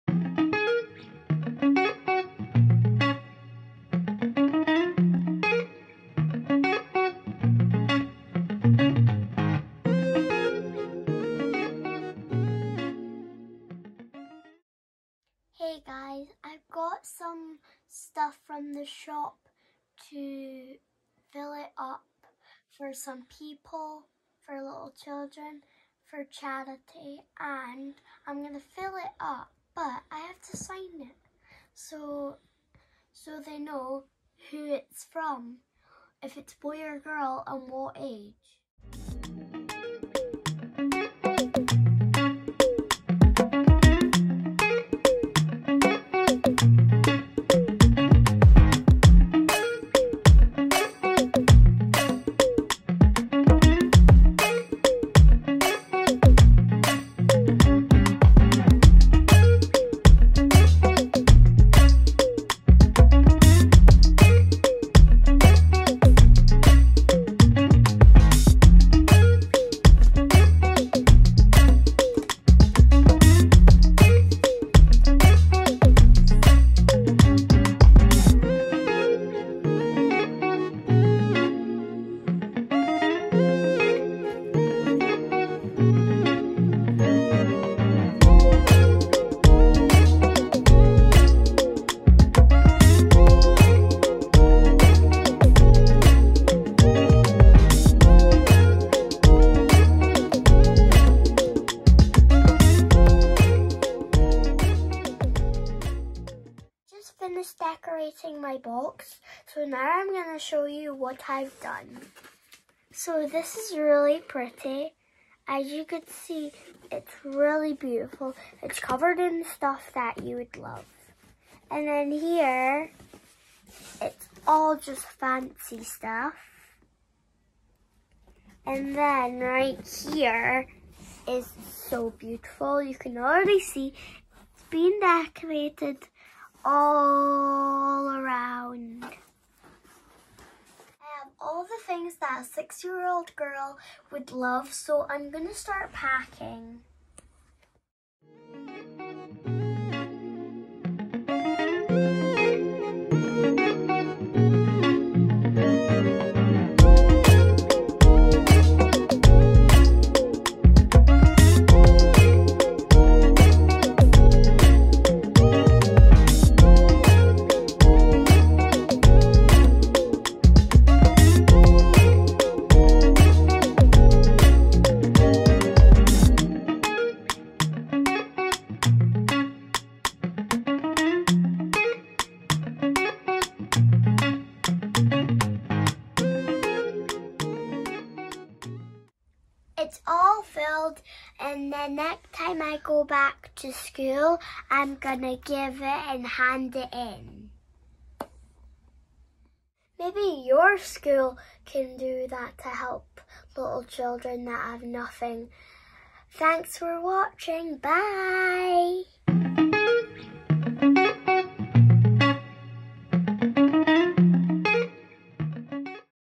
Rotary Shoebox Scheme
Lucy’s school are helping with the Rotary Shoebox Scheme, sharing a little happiness in Eastern Europe.

She had so much fun decorating and picking out special things for a 6 year old girl just like her ❤️